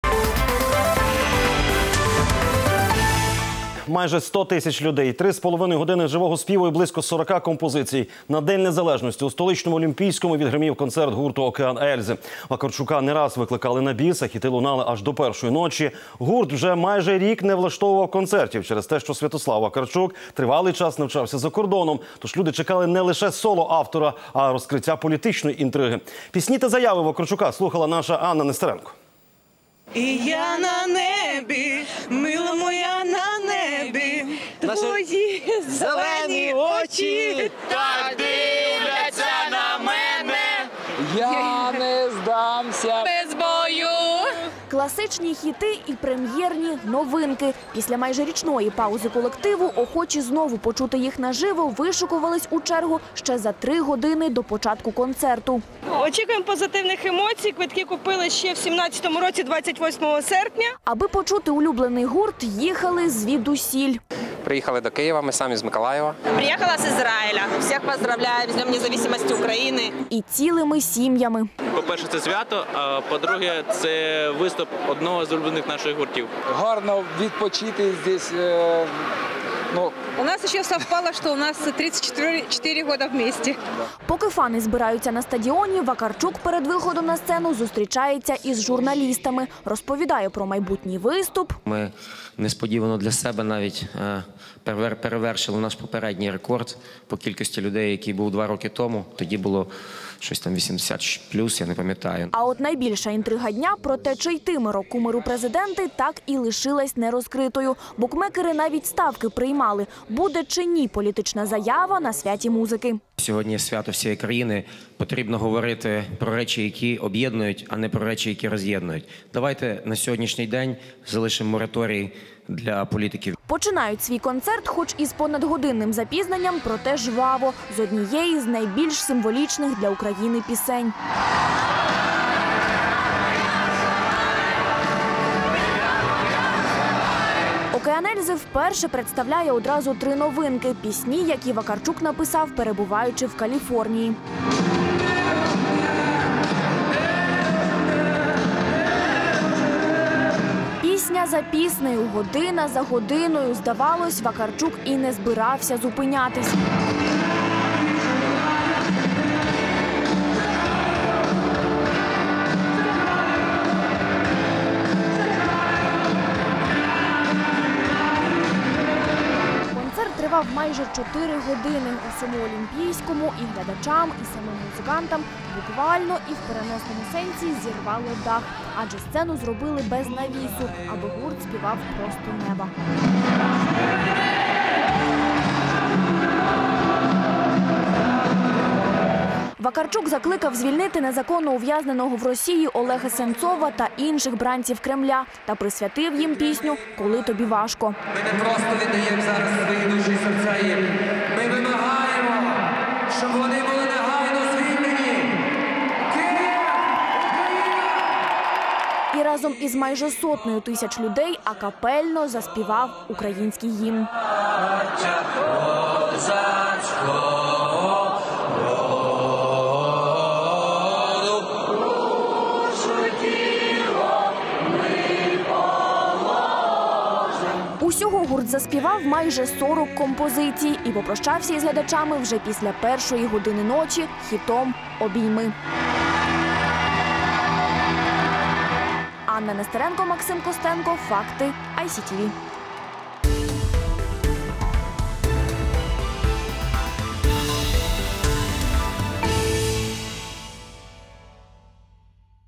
100 тысяч, 4 часа и 40 хитов: Океан Эльзы зажег Олимпийский на День независимости
Концерт длился почти 4 часа. Всему Олимпийскому - и зрителям, и самим музыкантам, буквально и в переносном смысле - сорвало крышу! Ведь сцену сделали без навеса - чтобы группа пела под открытым небом.

Все самые свежие и актуальные новости Украины и мира читайте ЗДЕСЬ ➡️